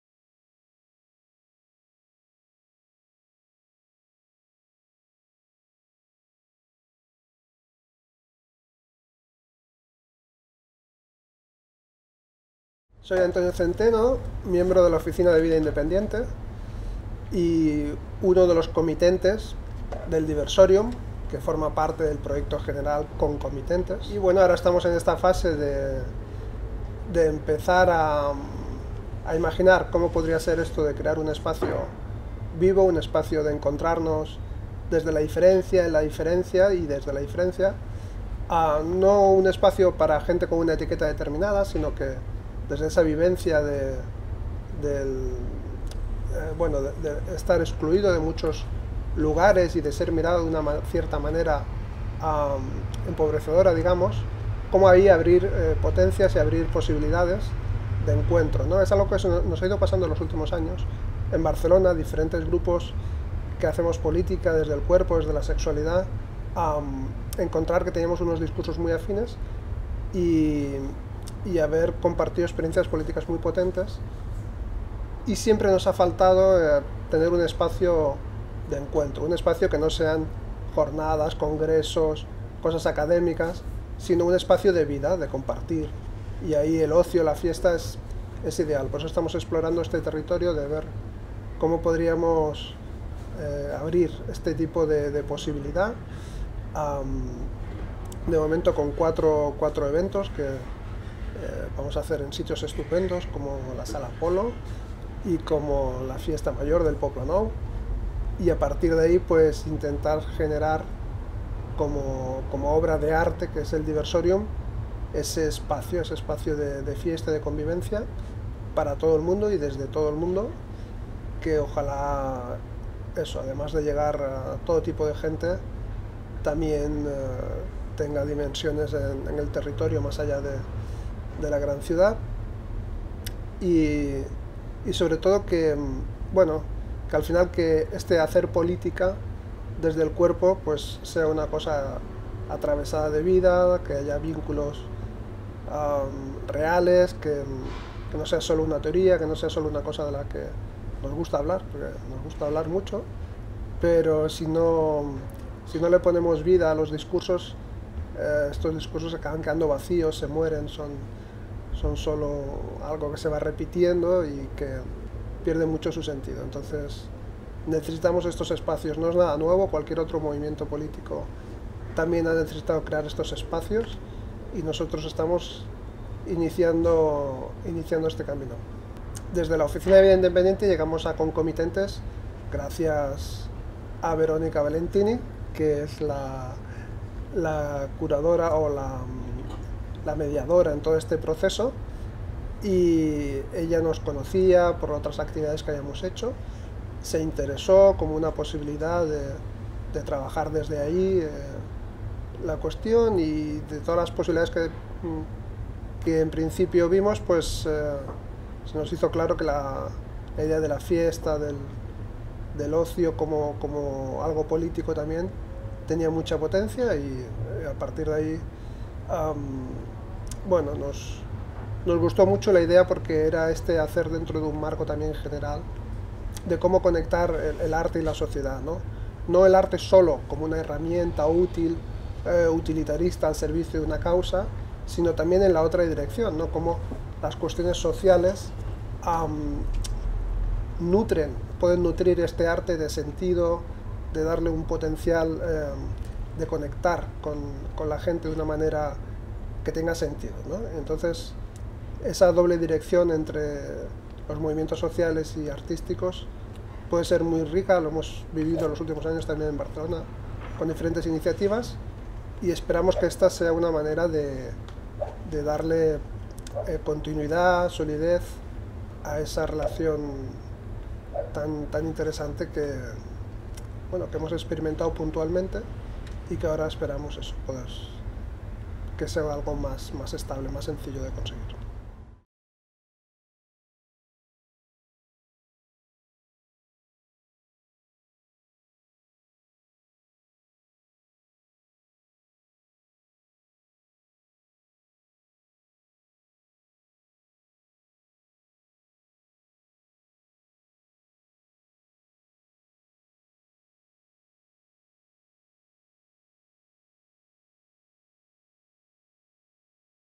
Diversorium_ Entrevista con el comitente Antonio Centeno
El alma de un proyecto son las personas que lo ejecutan. En el caso de nuestra concomitancia de Barcelona, Diversorium, estas personas son los comitentes, Antonio Centeno y María Oliver, integrantes de la Oficina de Vida Independiente de Barcelona (OVI).

Ellos entienden la cultura no sólo como un canal de difusión para discursos e idearios políticos, sino como un elemento real que incentiva los cambios en una sociedad. De la mano de la mediadora, Veronica Valentini, trabajan para promover un encuentro de todos esos cuerpos que quieren enriquecer su realidad a través del intercambio.

En esta entrevista, Antonio Centeno, relata su experiencia como comitente en este proyecto que se inició en 2018 y culminó en 2022.

Créditos:

Comitentes: Antonio Centeno
Grabación: David Ortiz
Edición y postproducción: Lorda Crusellas
Colaboradores: E.M.M.A
Mecenas fundador: Fundación Daniel y Nina Carasso